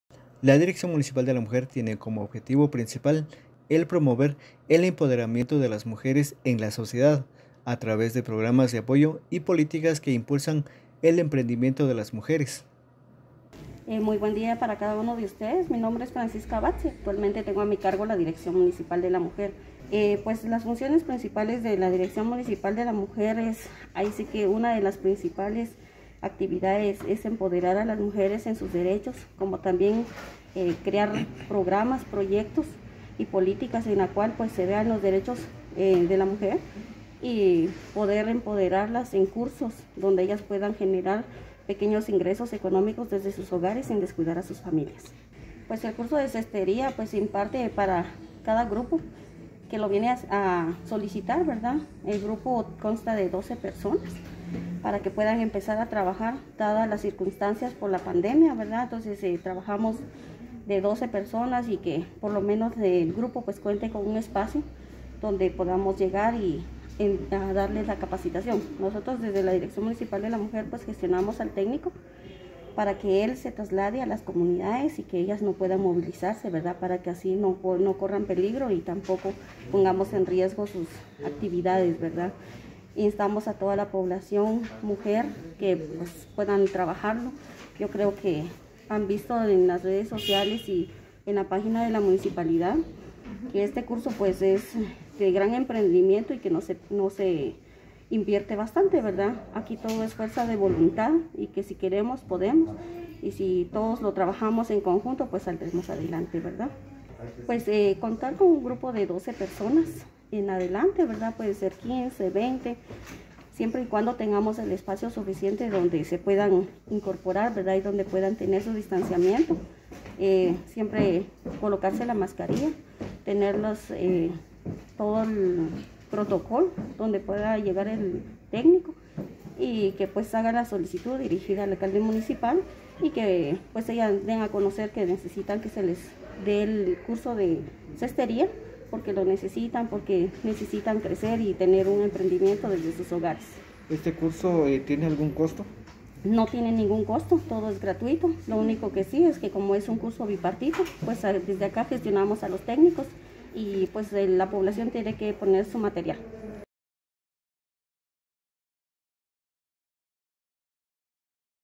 La Dirección Municipal de la Mujer fomenta emprendimiento en la población
La Dirección municipal de la Mujer tiene como objetivo principal el promover el empoderamiento de la mujer en la sociedad a través de programas de apoyo y políticas que impulsan el emprendimiento de las mujeres